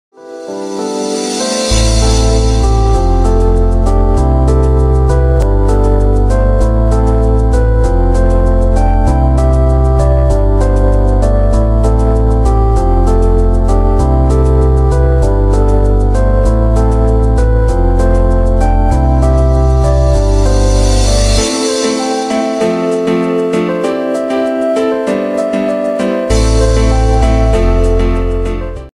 PRIMER CONSEJERÍA DE MUJERES EN SAN RAFAEL
En el marco del día internacional de la eliminación de la violencia contra la mujer, el municipio inauguró hoy la primer consejería para mujeres en la delegación de Salto de las Rosas.